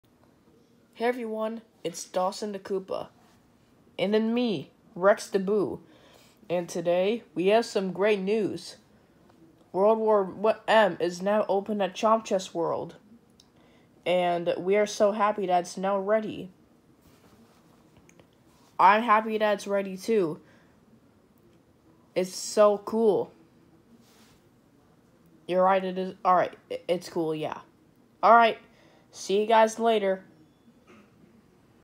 A message from Dawson and Rex
Rex the Boo and Dawson the Koopa, the stars of the new attraction World War M, gives some exciting news!